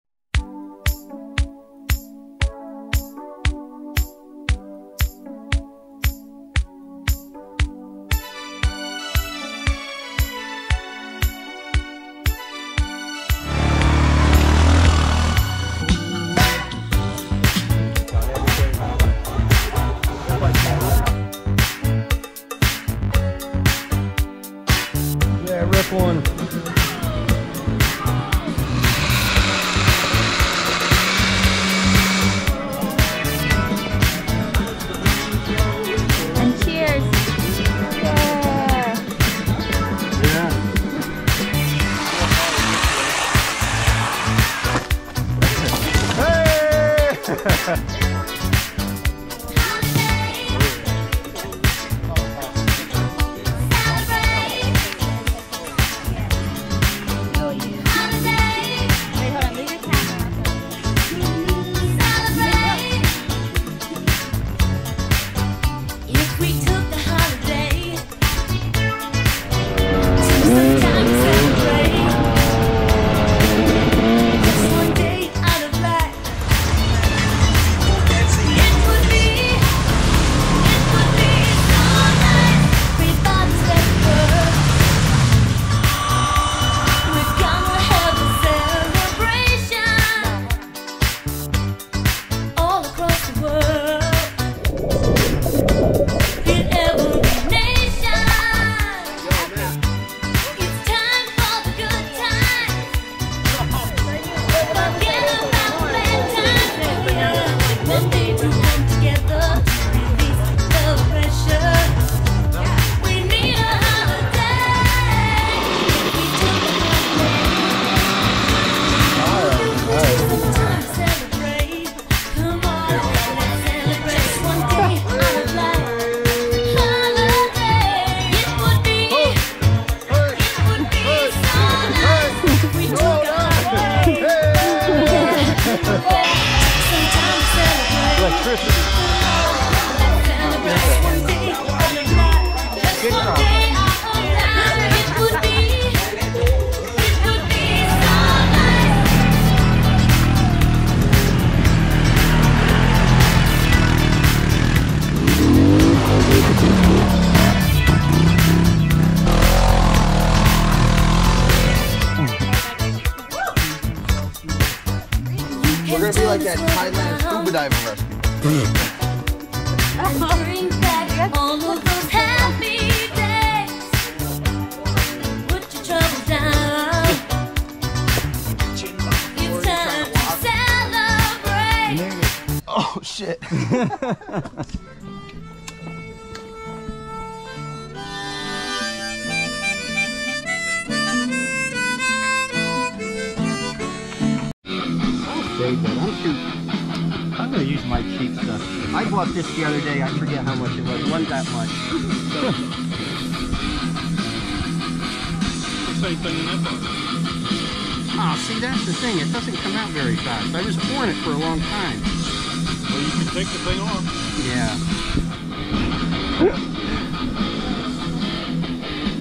bottle service blender party on quads and to secret cave !
awesome party